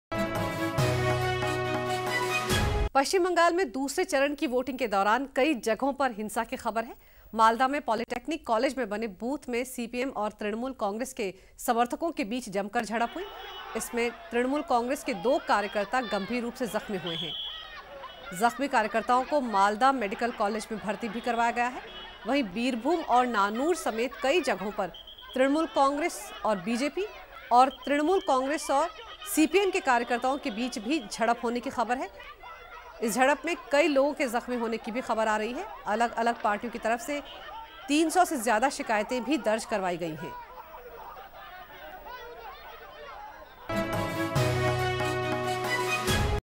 Violence during voting in West Bengal assembly polls
CPM-TMC supporters clashed in Malda during second phase voting in West Bengal assembly polls...